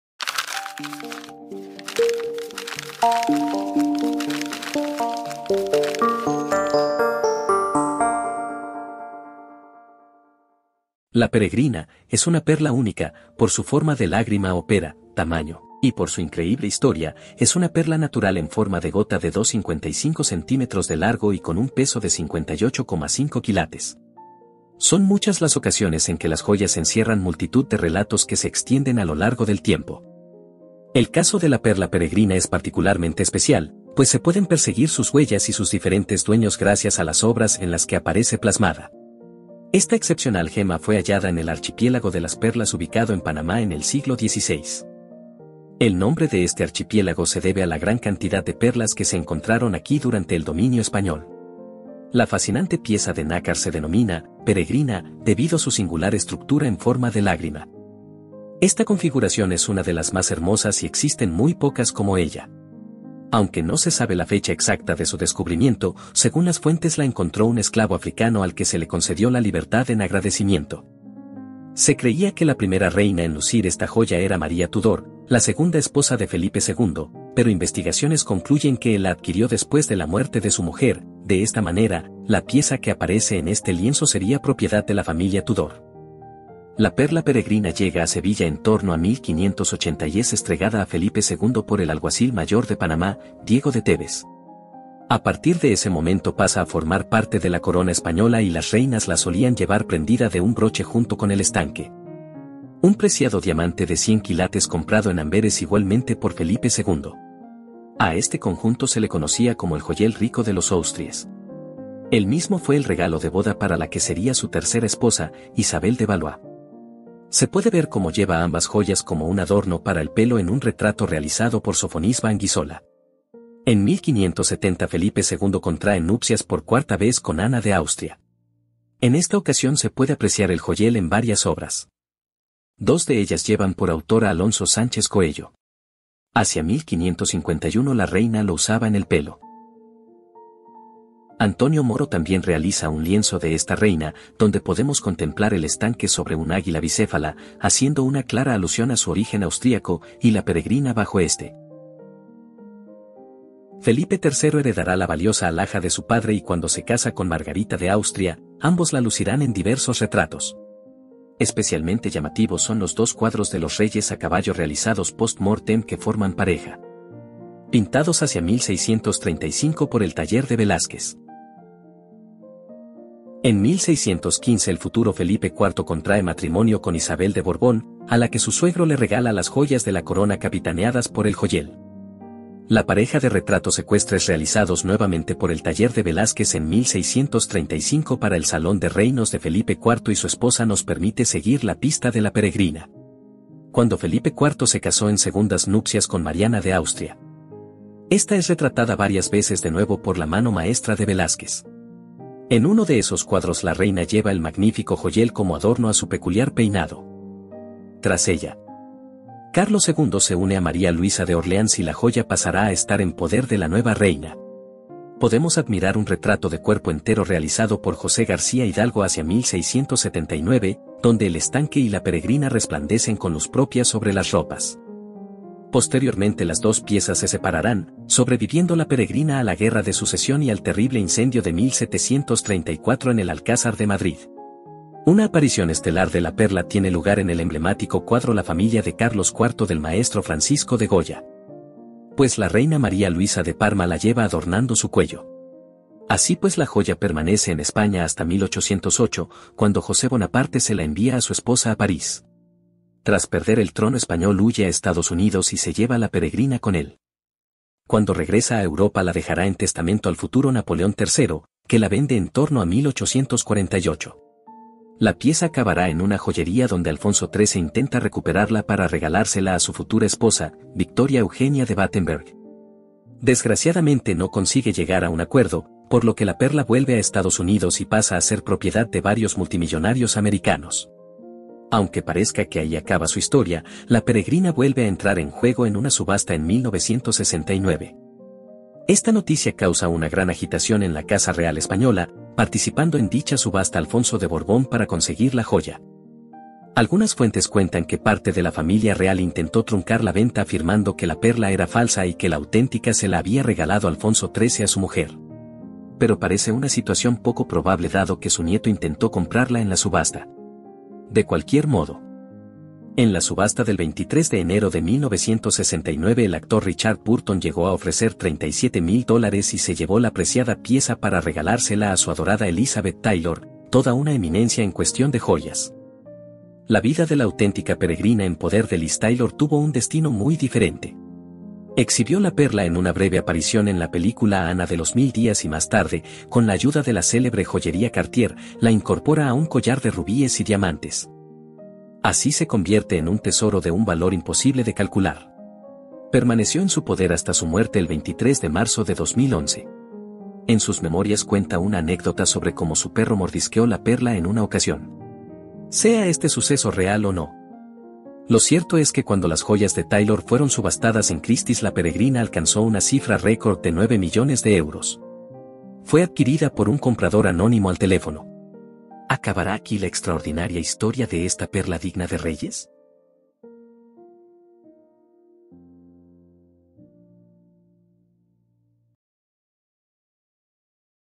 La Perla Peregrina, La historia de una de las joyas más famosa del mundo
La Perla Peregrina es una de las perlas más famosas y valiosas del mundo. Se cree que fue descubierta en el siglo XVI en las aguas del Caribe y llevada a España. La perla tiene una forma inusual de pera o lágrima y es de un tamaño excepcionalmente grande, midiendo alrededor de 31,6 milímetros de largo y 19,8 milímetros de ancho.

La Perla Peregrina ha sido propiedad de muchas personalidades históricas famosas, incluyendo la reina Mary I de Inglaterra, el rey Felipe II de España, y la actriz Elizabeth Taylor. Durante su tiempo en posesión de la reina Mary I, la perla fue montada en una joya llamada "La Peregrina", un collar que también incluía diamantes y rubíes.

En 2011, la Perla Peregrina fue subastada por Christie's en Nueva York y vendida por un precio récord de 11.8 millones de dólares a un comprador anónimo.